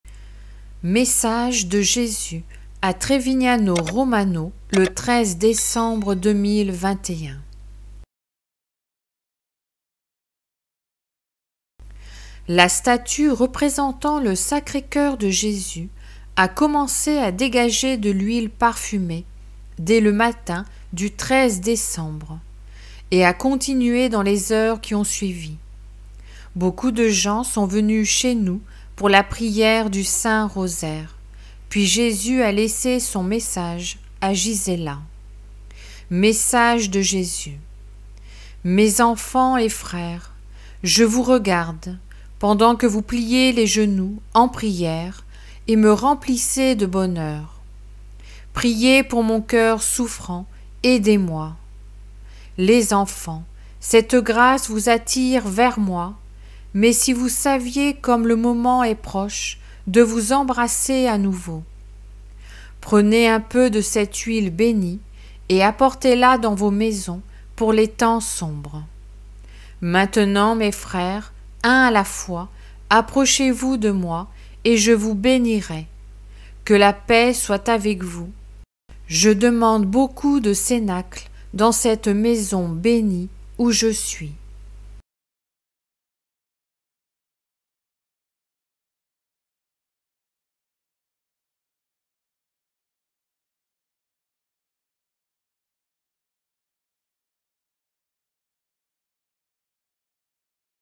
TREVIGNANO ROMANO ✨ MESSAGE de NOTRE SEIGNEUR JÉSUS CHRIST du 13 décembre 2021
📜Préparons-nous pour l'avertissement et l'illumination des consciences - Voici un examen de conscience pour faire une confession complète de sa vie
Message du Seigneur donné à John Leary : "L’Avertissement sera une illumination de votre conscience qui vous montrera le bon du mauvais sans aucune rationalisation. Tout le monde aura cette expérience surnaturelle hors du corps en même temps et où vous serez amené devant moi dans ma Lumière. Vous aurez une révision de vie montrant toutes vos bonnes et mauvaises actions avec une emphase sur vos péchés non pardonnés et vos péchés d’omission.
À la fin de cette révision de vie, vous verrez où vous serez jugés d’aller, soit au ciel, au purgatoire ou en enfer. Puis vous aurez un petit aperçu de ce que se sera à l’endroit où vous aurez été jugés d’aller.
Ensuite, vous serez ramenés dans votre corps, et grâce à ma miséricorde, il vous sera donné une seconde chance de mener une meilleure vie."

✨Message de Notre Seigneur Jésus Christ à Trevignano Romano le 13 décembre 2021 - Message du ciel pour toi aujourd'hui du Seigneur Jésus - Message prophétique 2021 donné à Gisella Cardia  en Italie

🙏 Prière de communion spirituelle: Sainte Marie, Mère de Dieu et Notre Mère Sanctuaire Vivant du Dieu Un et Trine. Donne-nous ton Fils Jésus, le Fruit Béni et prie pour nous si pécheurs…

Merci

N'oubliez pas d'activer la CLOCHE de NOTIFICATIONS 🔔